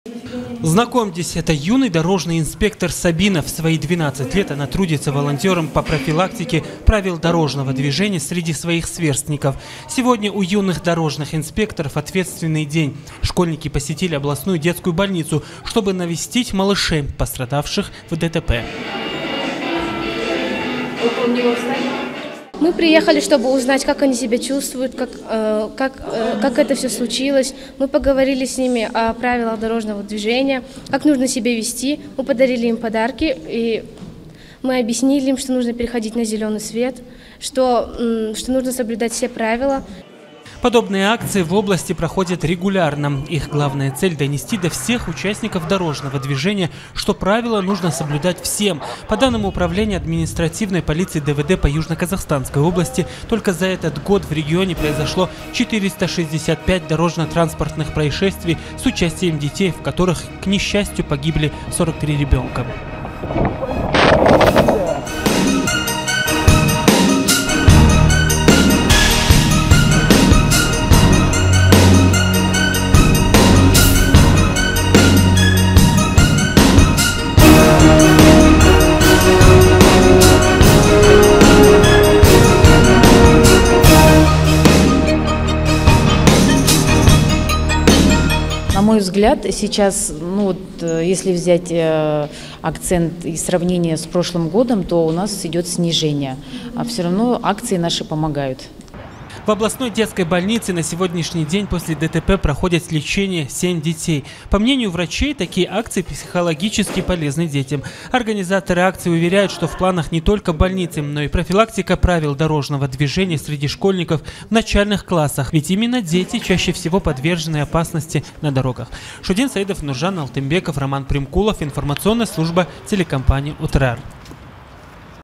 Юные дорожные инспекторы сегодня навестили пострадавших в ДТП детей
Юные дорожные инспекторы сегодня навестили пострадавших в ДТП детей в детской областной больнице. Юные полицейские с игрушками и буклетами рассказали о правилах дорожного движения, о том, как нужно вести себя на дороге и пожелали пациентам больницы скорейшего выздоровления. Такие акции проводятся сотрудниками административной полиции регулярно.